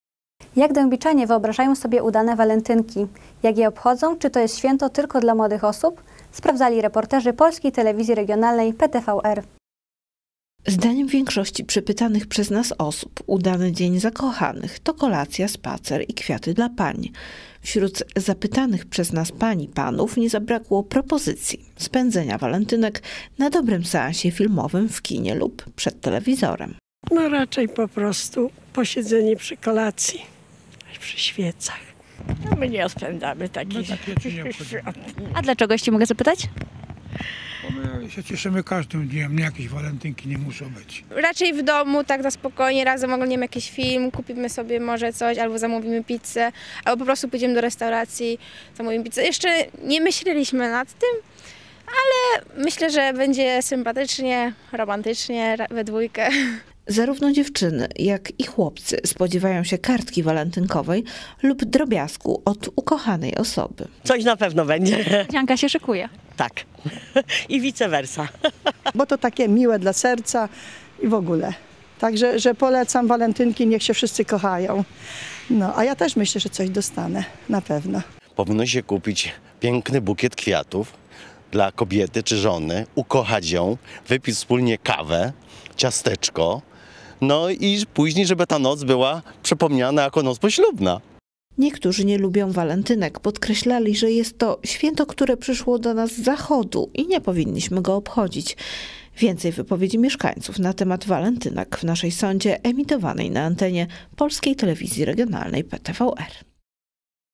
Przepis na udane Walentynki
Jak dębiczanie wyobrażają sobie udane Walentynki?, jak je obchodzą, czy jest to święto dla tylko młodych osób ?– sprawdzali reporterzy Polskiej Telewizji Regionalnej PTVR.

Zdaniem większości przepytanych przez nas osób udany Dzień Zakochanych to kolacja, spacer i kwiaty dla pań. Wśród zapytanych przez nas pań i panów nie zabrakło propozycji spędzenia Walentynek na dobrym seansie filmowym w kinie lub przed telewizorem.

Zarówno dziewczyny jak i chłopcy spodziewają się kartki walentynkowej lub drobiazgu od ukochanej osoby. Niektórzy nie lubią Walentynek. Podkreślali, że jest to święto, które przyszło do nas z zachodu i nie powinniśmy go obchodzić. Więcej wypowiedzi mieszkańców na temat Walentynek w naszej sondzie emitowanej na antenie Polskiej Telewizji Regionalnej PTVR.